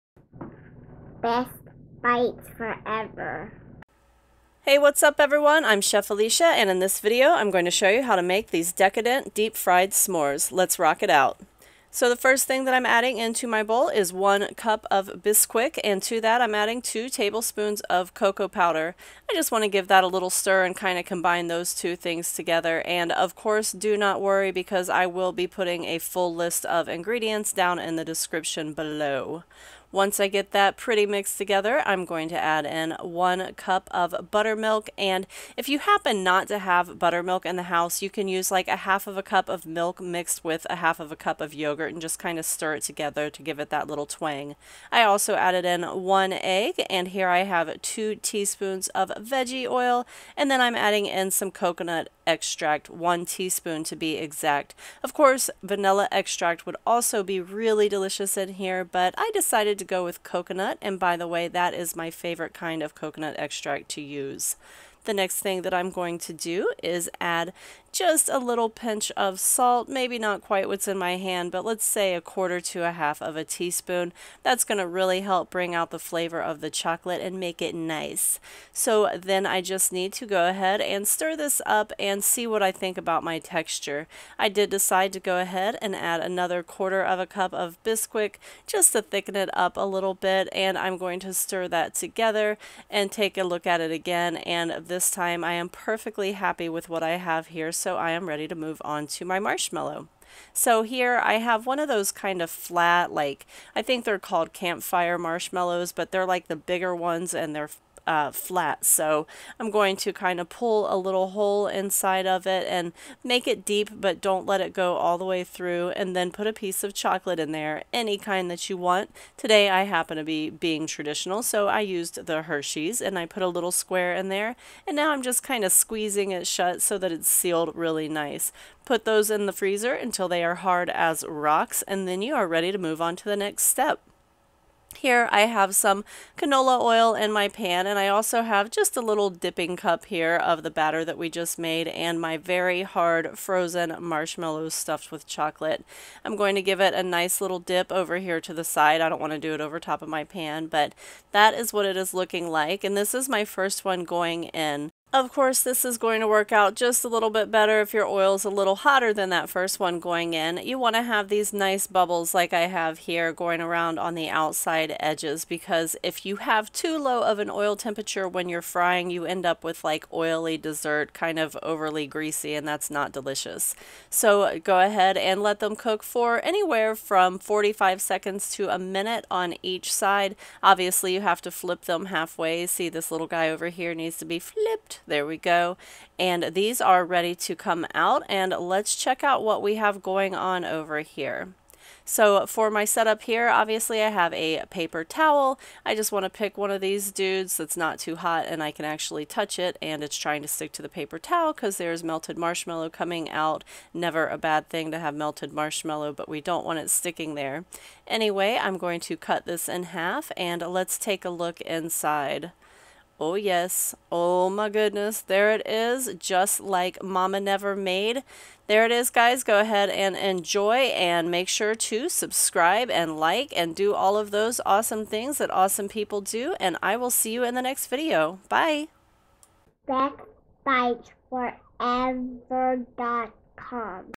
S'mores - S'mores Recipe - Deep fried S'mores
S'mores are a classic campfire dessert and in this s'mores recipe, I'm putting a twist on them by deep frying them - mmmm, deep fried s'mores. Yum!